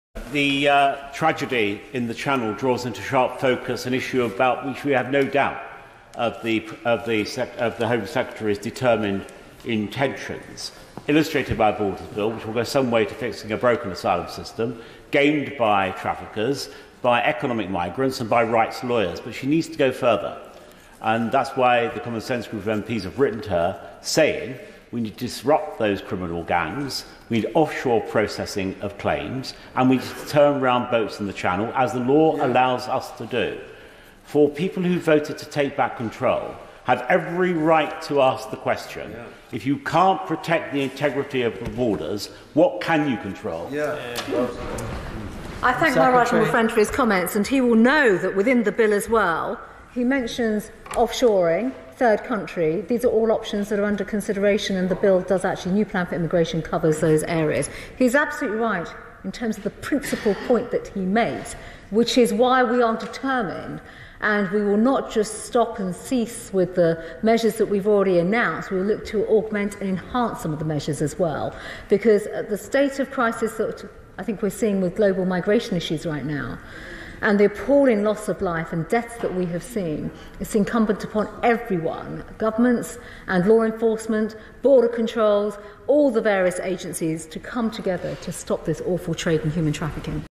John Hayes: "If you can't protect the integrity of the borders, what can you control?"
"People voted to take back control.'

"It's incumbent upon everyone," says Home Secretary Priti Patel "to stop this awful trade in human trafficking."